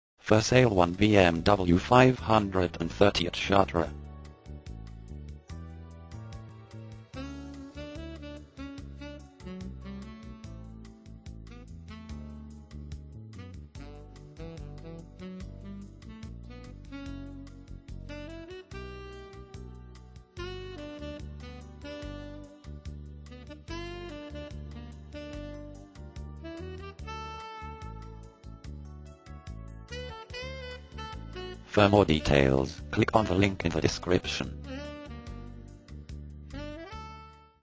Sale one BMW 530 Chartres Eure-et-Loir
For sale at  Chartres  Used car BMW 530 of 199000 Km.
used motors Autoenvideo : Sale one BMW 530  Chartres  Eure-et-Loir